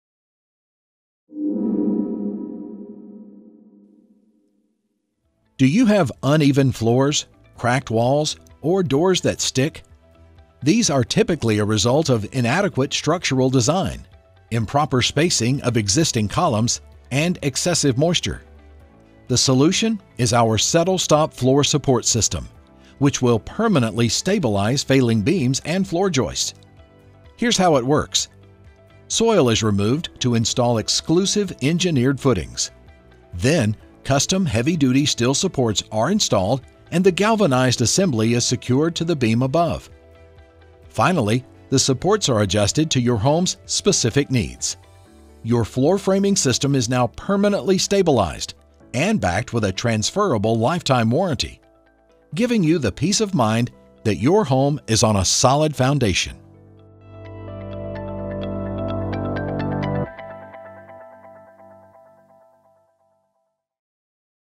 Permanently Stabilize Your Home's Floor Framing System
The floor framing system in your crawl space is designed to support the weight of the floors and walls above. Over time, this system can weaken causing the load bearing beams and floor joists to sag. The SettleStop Floor Support System will permanently stabilize the failing beams and floor joists. Unlike other traditional methods, the unique system can handle vertical loads exceeding 50,000 pounds, insuring stable floors and walls.